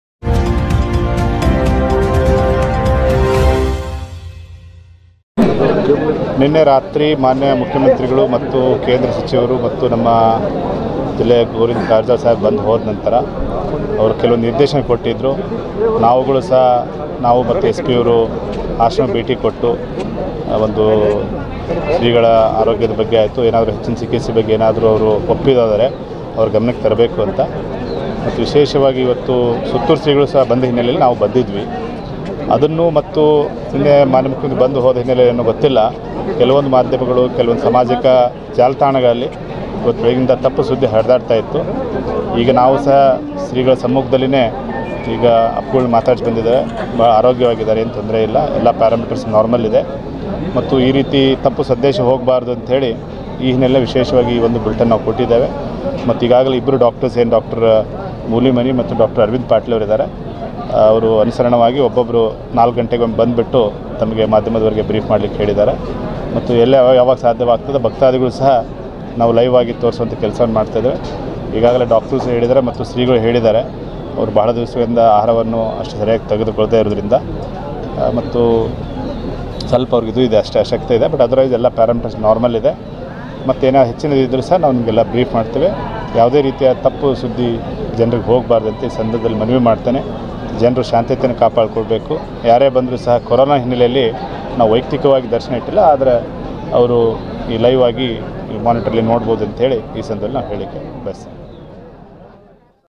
SIDDHESHWAR SWAMI JI KE TABIYATKE BARE MEIN BIJAPUR KE DC NE | T NEWS EXPRESS 01-01-2023
SIDDHESHWAR SWAMI JI KE TABIYAT
KE BARE MEIN BIJAPUR KE DC NE